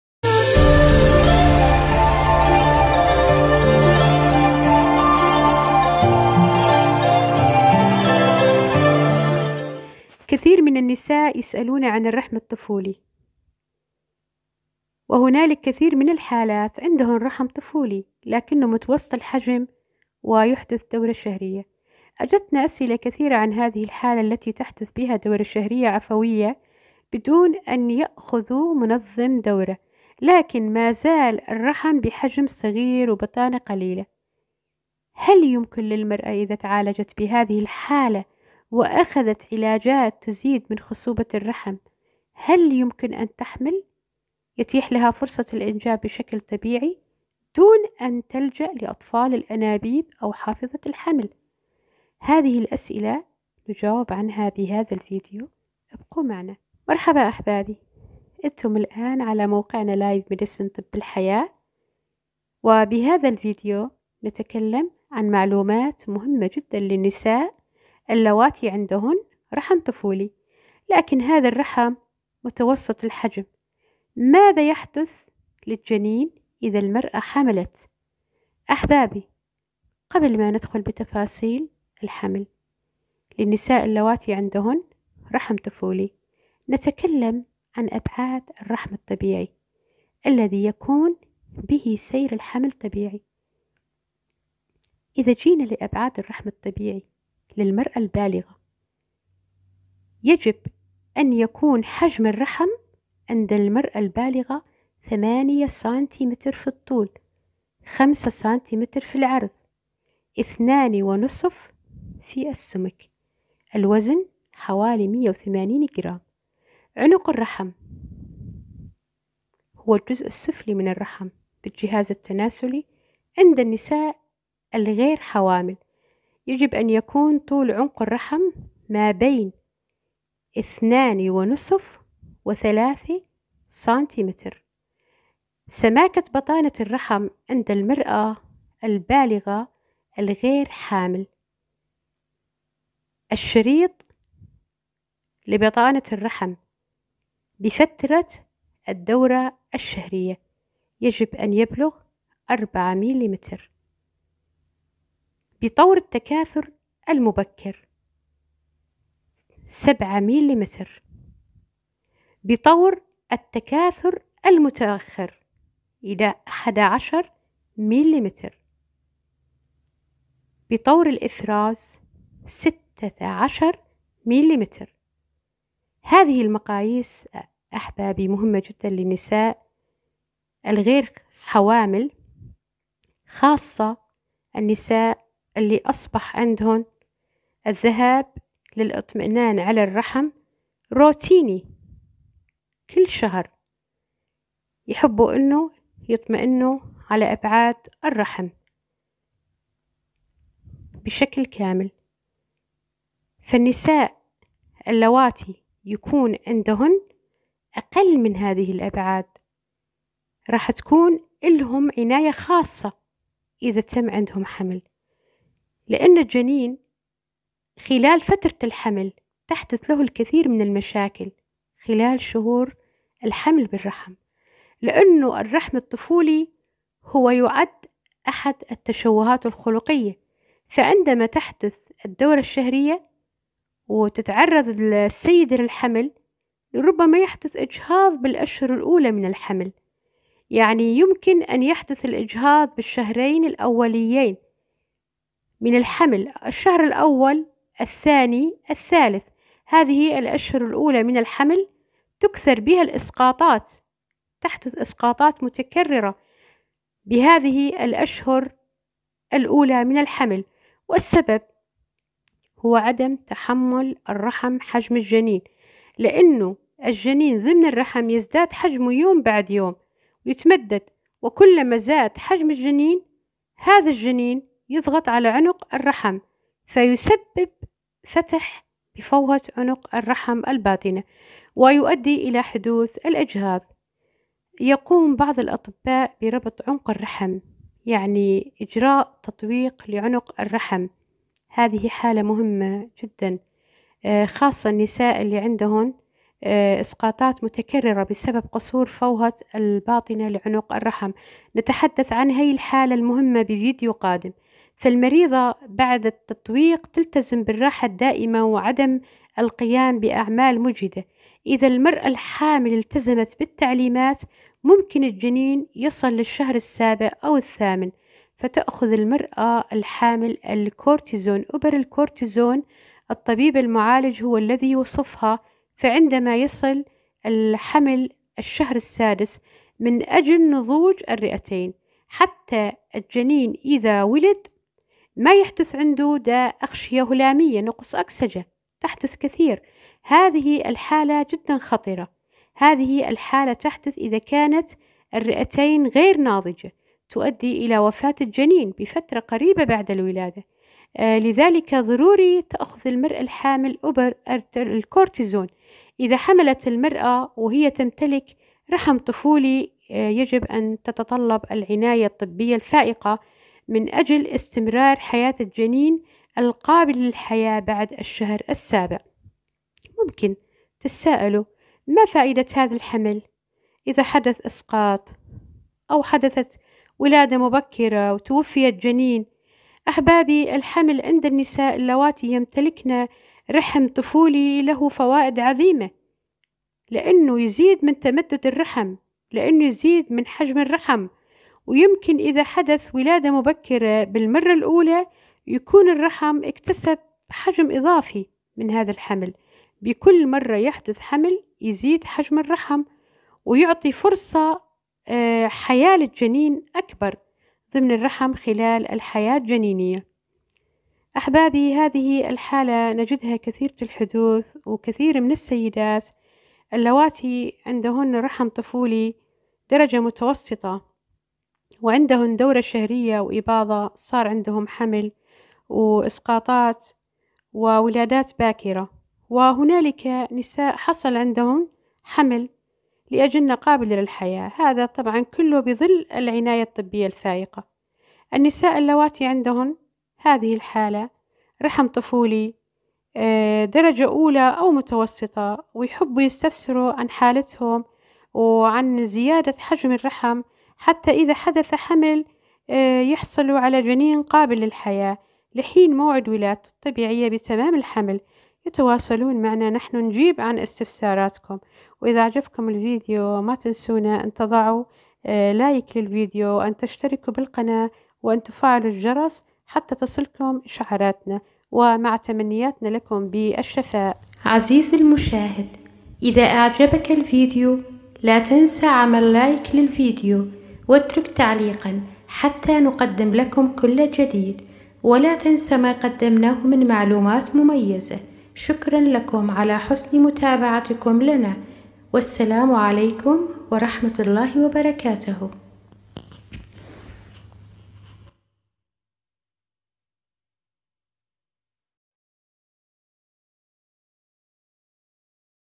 هام جدا للنساء هل يمكن للمرأة التي تمتلك رحم طفولي ان تحمل بدون بدون زرع او حاضنة الحمل- طب الحياة
male pregnancy
baby
small uterus